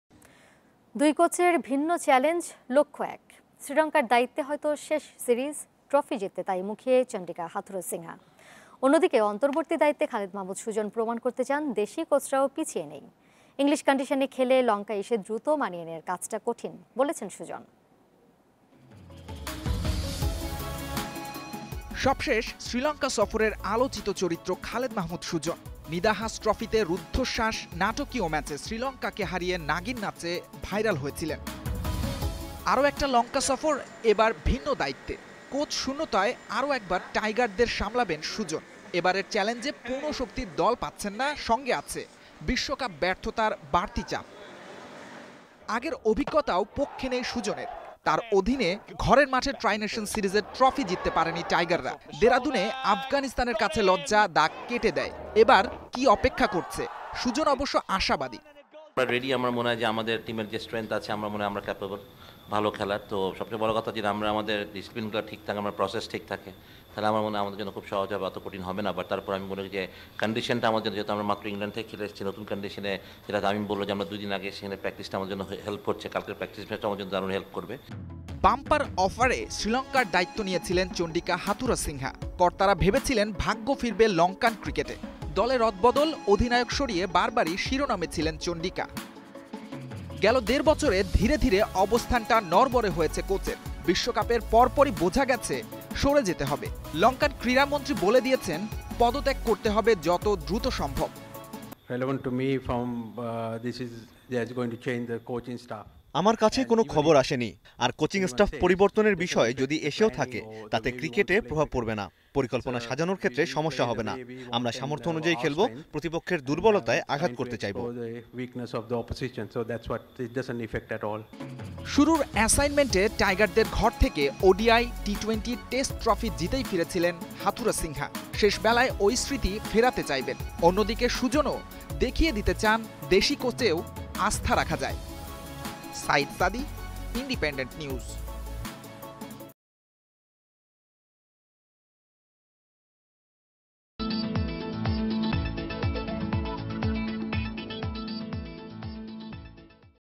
এই সিরিজই কী হাথরুসিংহের শ্রীলংকান কোচিংয়ের শেষ অধ্যায়? || Bangladesh vs Sri Lanka
দুই কোচের ভিন্ন চ্যালেঞ্জ, লক্ষ্য এক। শ্রীলঙ্কার দায়িত্বে হয়ত শেষ সিরিজ, ট্রফি জিততে তাই মুখিয়ে চন্ডিকা হাথুরুসিংহা। অন্যদিকে অন্তর্বর্তী দায়িত্বে খালেদ মাহমুদ সুজন প্রমাণ করতে চান, দেশি কোচরাও পিছিয়ে নেই। ইংলিশ কন্ডিশনে খেলে লঙ্কায় এসে দ্রুত মানিয়ে নেয়ার কাজটা কঠিন বলেছেন সুজন। অন্যদিকে লঙ্কান অধ্যায় এখনই শেষ হচ্ছে কিনা, তা নিয়ে রহস্য রেখে দিয়েছেন হাথুরুসিংহা।